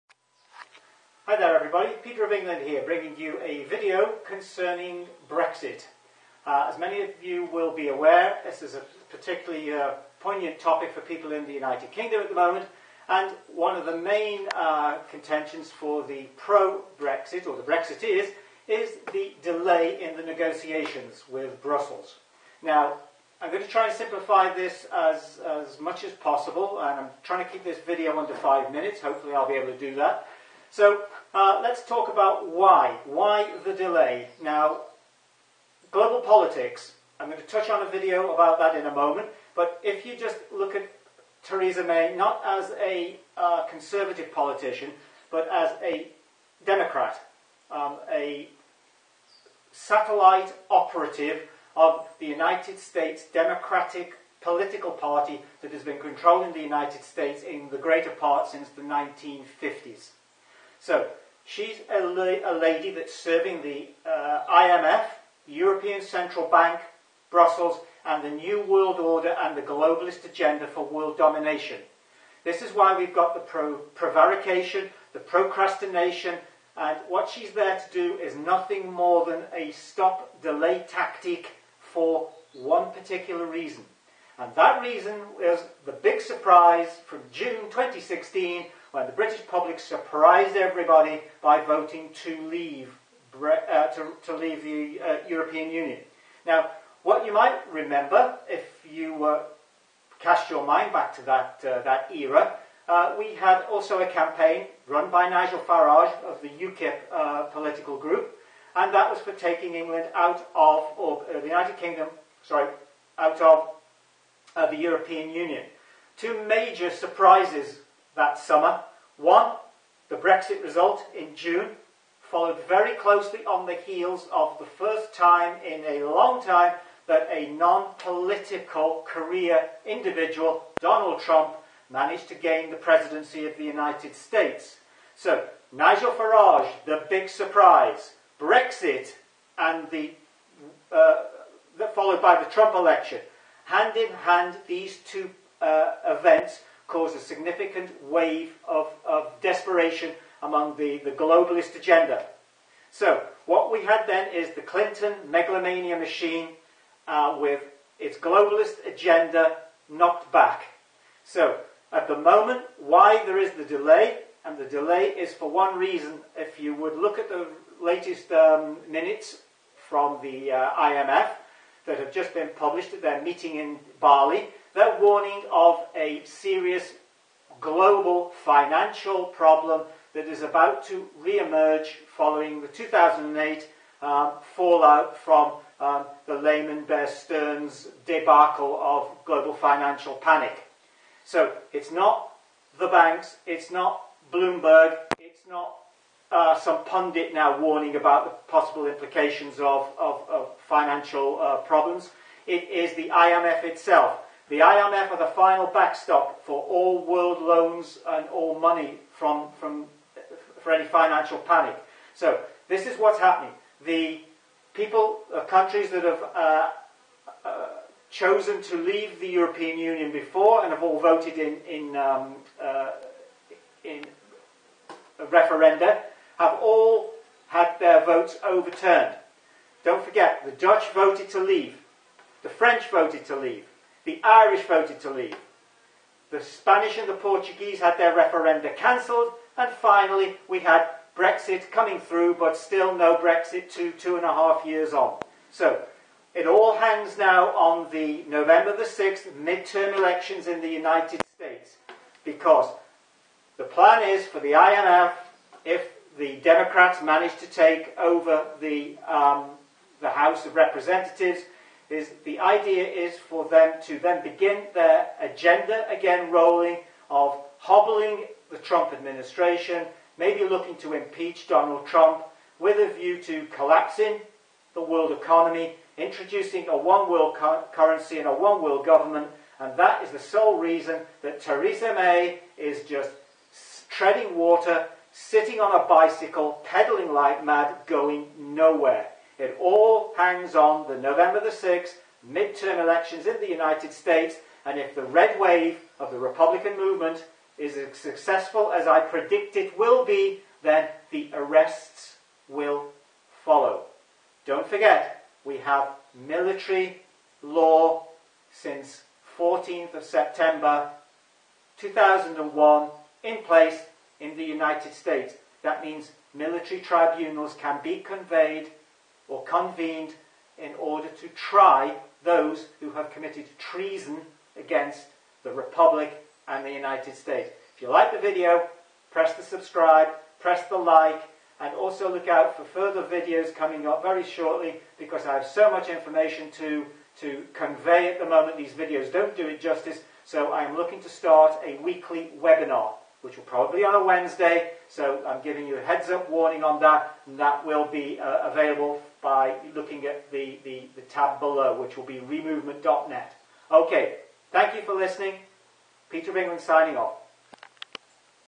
Theresa May is a Demon-o-crat, Satanist Worshipping Lackey. Fact!
May is under instruction to tread water in the hope that the Demon-o-crats can retake the Senate [ post Nov 6th 2018] and then begin to re-install themselves. Re-installing means beginning to stymie all of the Trump legislation in place. They will open the Mexican border and unleash these Nike, T-shirt wearing, fat baseball cap wearing hordes which the Demons have paid to travel the frontier.

Does anyone out there think that it takes 3 years to leave the EU?
You are being totally arsed about...big time, with stories of imminent catastrophe should an agreement not be sweet enough.

The Plan - Demonic
The IMF is set to unleash hell with the introduction of the SDR following a staged (finally) global economic melt-down blamed partly on the China Trade War and the festering toxic fall out  of the  of 2008 financial crisis debacle! This is the tipping moment. BUT the Demon-o-crats have to retake the Senate and the control both houses to do this!

If they fail - they then will also unleash a nasty series of "Armageddon" like final shots at the world which could include a "false flag" ET visitation engineered by the Rogue Deep State element. Why do you think "The Donald" has budgeted  to create the "Space Force?"
And who's paying for it? Thanks MBS - Crown Prince, "That'll do nicely!"

So Merkel, May, Macron and Brussels stand by awaiting the "good news" which will not come...but wait they will.

However, when the Red Wave ascends and the Republicans move to a massive majority (needed in the Senate) then the Military Tribunals can be activated. Current Senate  numbers are Rep.51, Demon-crats 49 and 2 Independents, which means if someone in the Rep. party has a cold then they have to wait their return in order to pass legislation.

So buckle up and prepare for the next 911 which will on on the 9th of November 2018, 3 days post Victory.